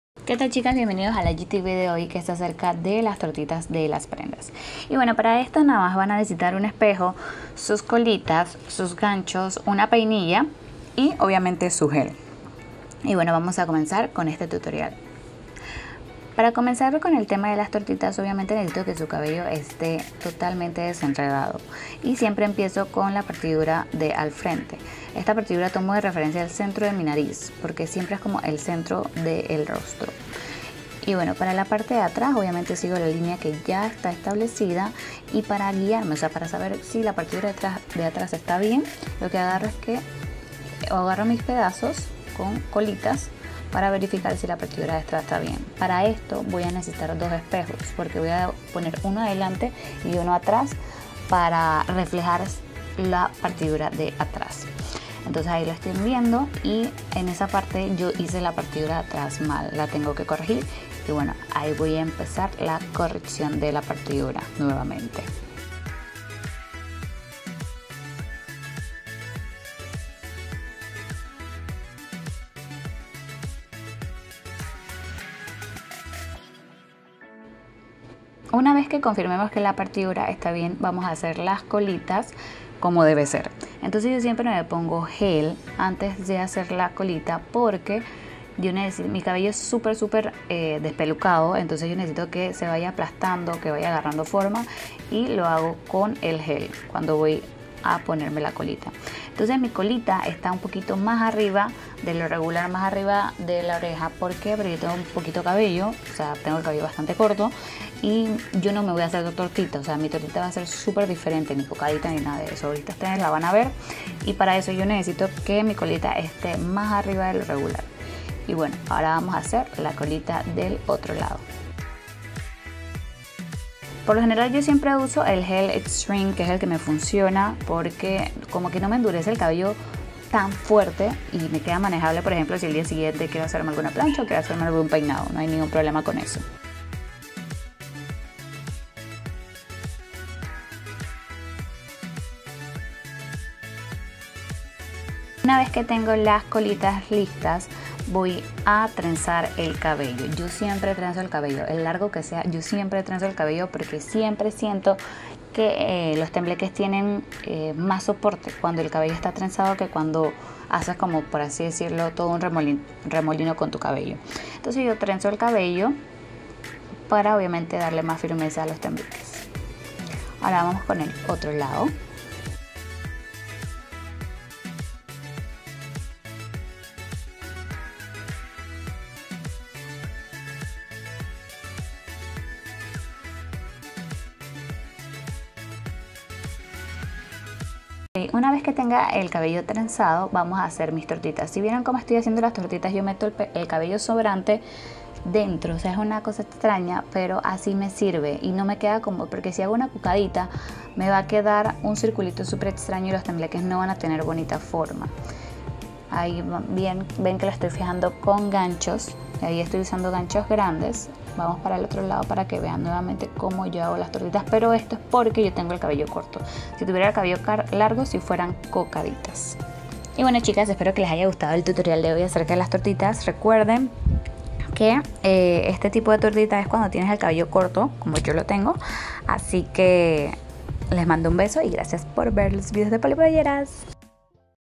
Aprende a hacerte las tortitas para que tus tembleques te queden bien!
Este vídeo es super practico para las chicas de cabello corto! Te enseñare como hacerlo!!!